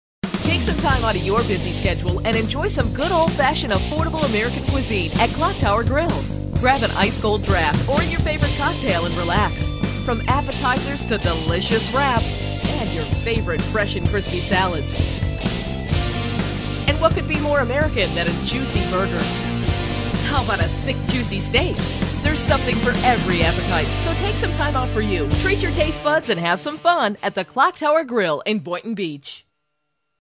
Clock Tower Grill, The Place To Be
There's nothing like affordable, good, old-fashioned American food, and there's no better place to enjoy it than Boynton Beach's Clock Tower Grill. Every satisfying dish here is made from scratch. Like your best friend, they're always here for you, seven days a week! The Clock Tower even has a special Menu from 9pm to 1am for those late night cravings.

From the tempting appetizers, Tick Tock Rock Shrimp, marinated and fried to a golden brown, with spicy Dynamite Sauce; Tex-Mex Egg Rolls; and piping hot Spinach and Artichoke Dip with fresh tortilla chips are standouts. You'll find all your favorite comfort foods here, like New England Clam Chowder, Tuna Melts, Philly Cheese Steak Sandwiches, healthy Wraps, cool salads, fat sandwiches -- with your choice of irresistible sides -- all freshly prepared to order. The delicious entrees represent every culinary region of our great country: Southern Fried Chicken Breast; crispy Long Island Duck a la Orange, Maryland style Crab Cakes What kid (or grownup) doesn't love pizza, thick milkshakes, Apple Cobbler and Carrot Cake? Enjoy a cold beer, wine or cocktail with your Karaoke, Beer Pong, live music and fabulous food. The Clock Tower Grill makes it delicious to be in the USA!